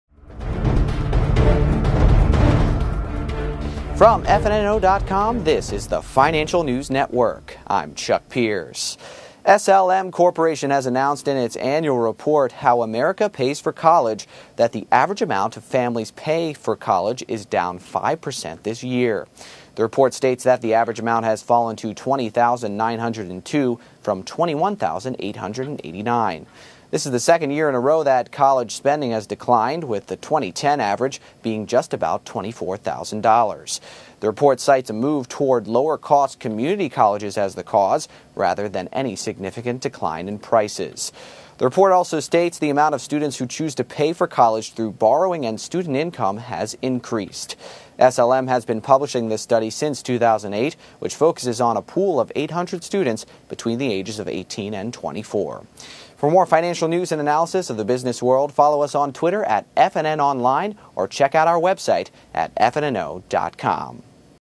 Americans Paying Less for College
SLM Corp. has announced in its annual report ""How America Pays for College"" that the average amount families pay for college is down 5% this year. The report states that the average amount has fallen to $20,902 from $21,889.

This is the second year in a row that college spending has declined, with the 2010 average being just about $24,000. The report cites a move toward lower cost community colleges as the cause, rather than any significant decline in prices.

The report also states the amount of students who choose to pay for college through borrowing and student income has increased. SLM has been publishing this study since 2008, which focuses on a pool of 800 students between the ages of 18 and 24.